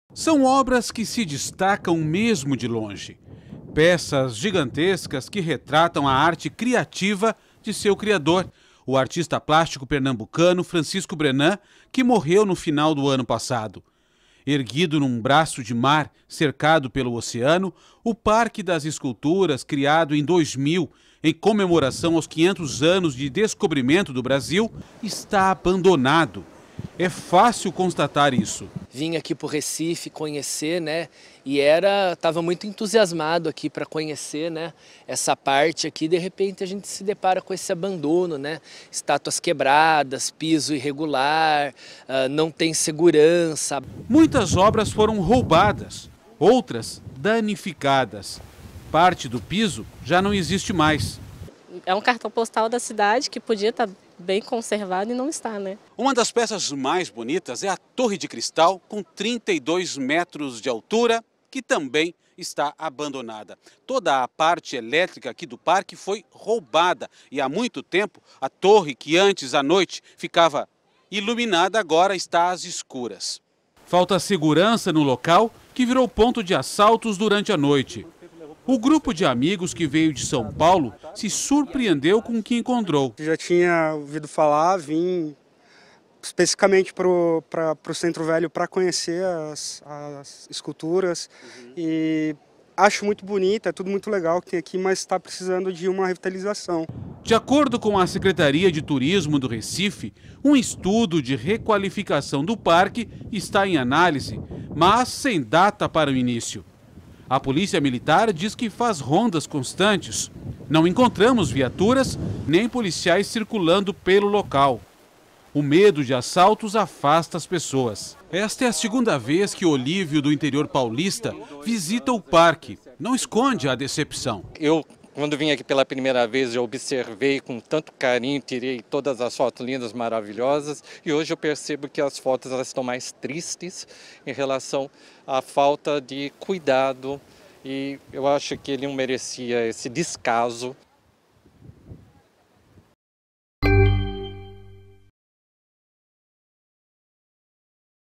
Abandono e insegurança no Parque das Esculturas de Francisco Brennand
Depredação e a falta de segurança são as principais reclamações de quem visita o Parque das Esculturas de Francisco Brennand, no bairro do Recife.